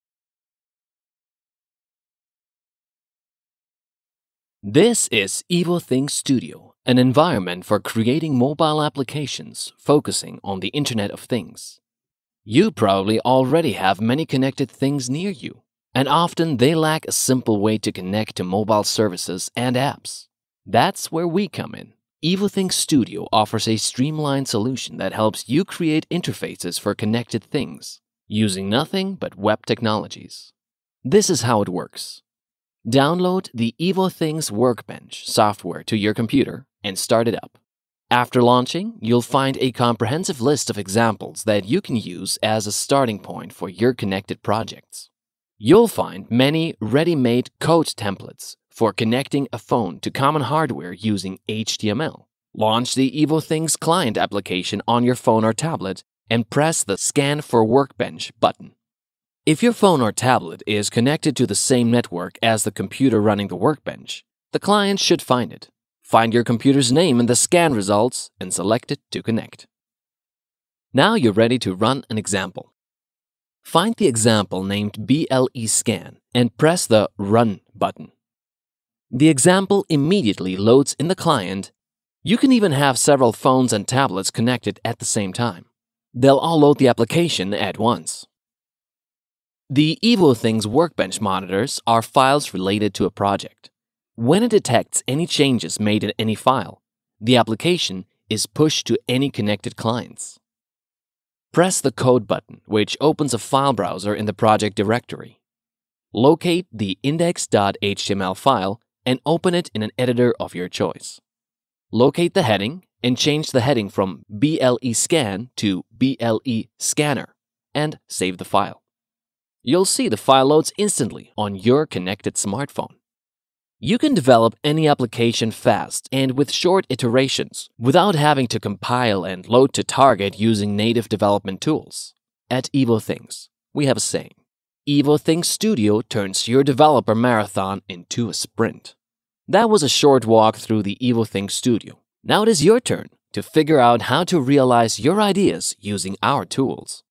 Evothings Studio -- making mobile IoT apps using web technologies
An environment for creating mobile applications focusing on the internet of things, found At Evothings, we strive to open up the mobile services and app space to new groups of developers, harnessing the power of mobile with the quick turnaround in development expected by companies and developers in the mobile space. With a tad bit of HTML5 knowledge, you can build apps using the Evothings Studio developer toolkit.